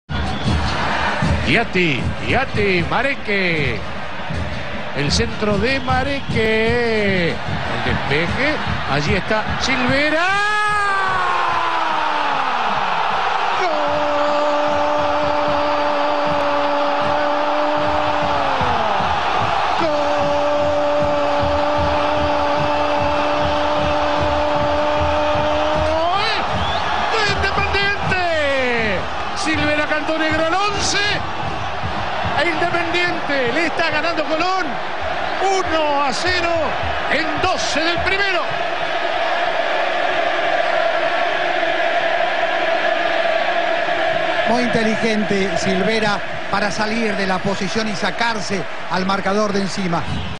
Andres Cuki Silvera - Independiente vs Colon - Apertura 2009
Gol de Cuki Silvera jugando para Independiente partido vs  Colon en el torneo Apertura del año 2009.